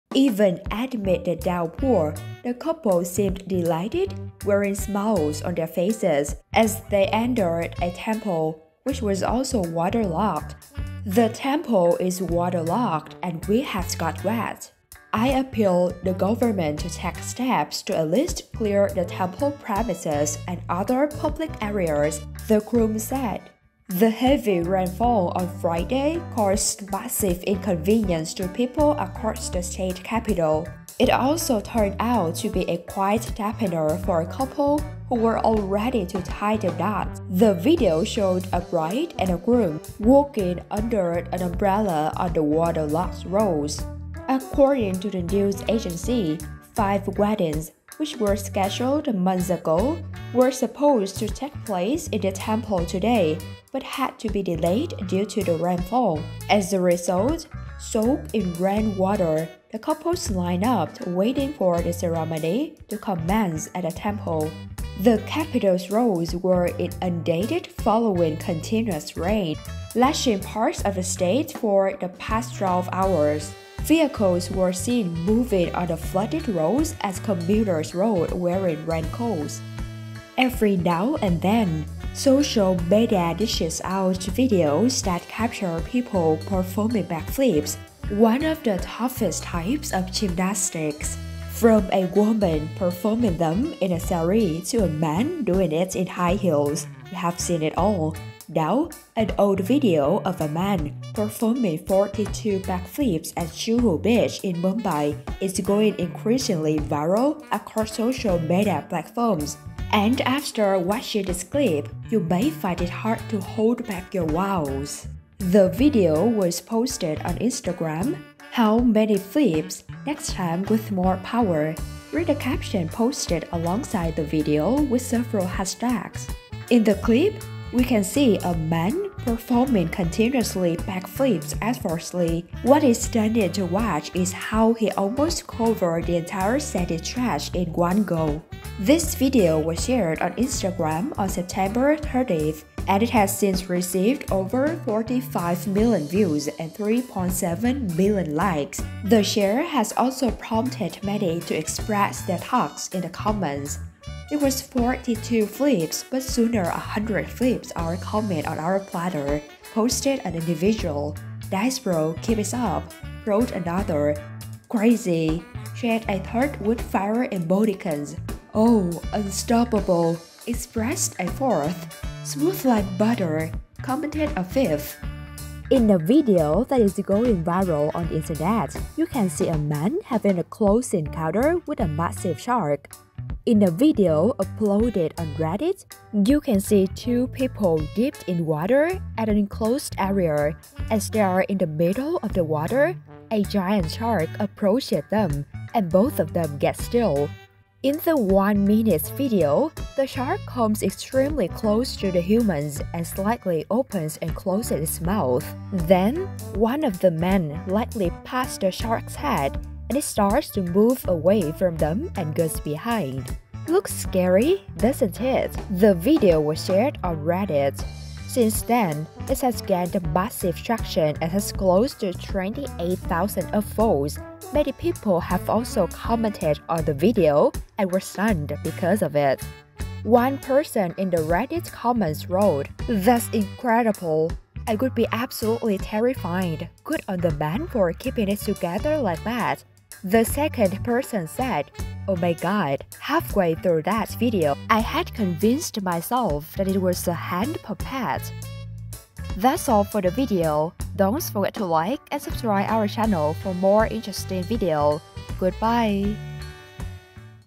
Incessant Chennai rain fails to deter couple ready to tie the knot | 3 Min News
The video shows a bride and a groom, drenched in rain, walking under an umbrella on the waterlogged roads in Chennai's Pulianthope area.